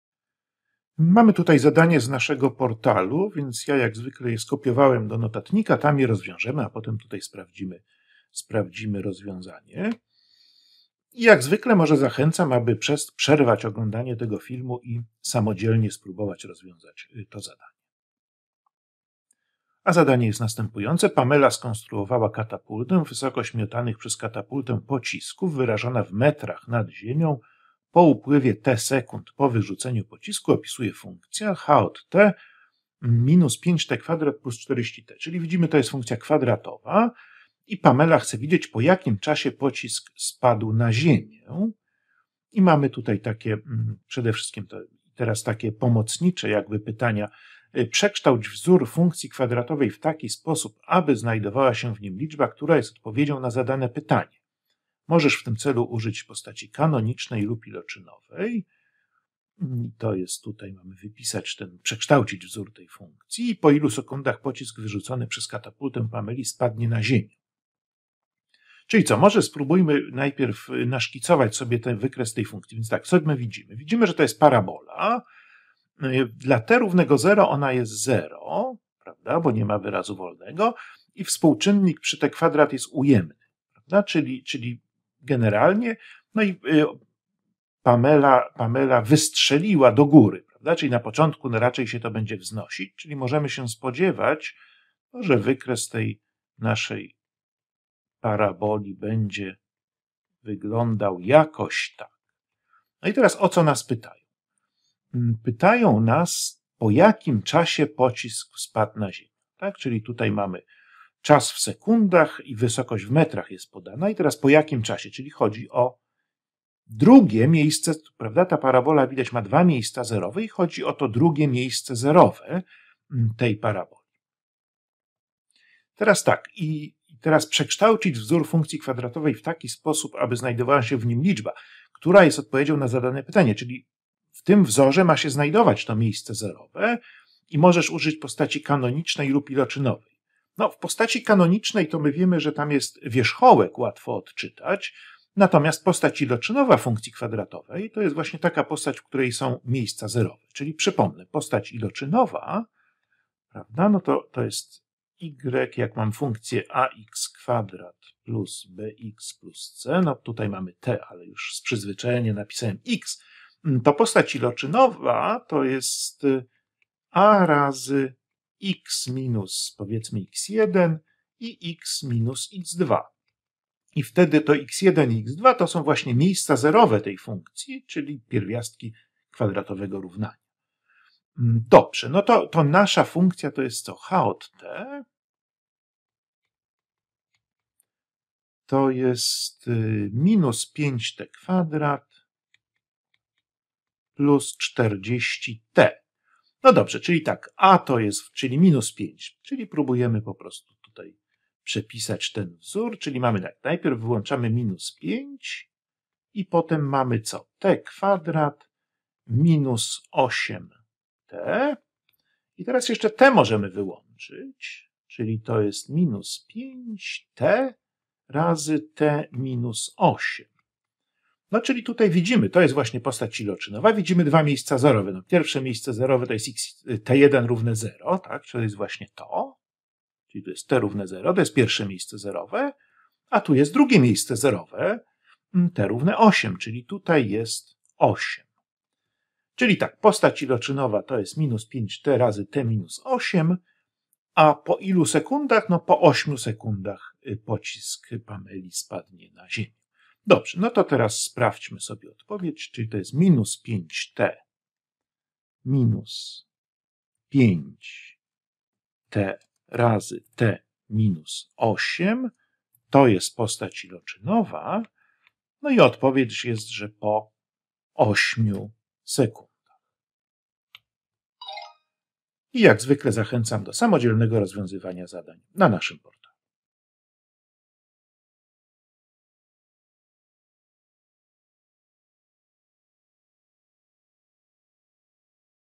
Interpretacja funkcji kwadratowej użytej w modelu - postać iloczynowa
Film na licencji CC: NC-BY-SA zrealizowany przez  Fundację Edukacja dla Przyszłości przy wsparciu Fundacji Akamai.
Opowiada Piotr Stachura z Instytutu Informatyki Technicznej SGGW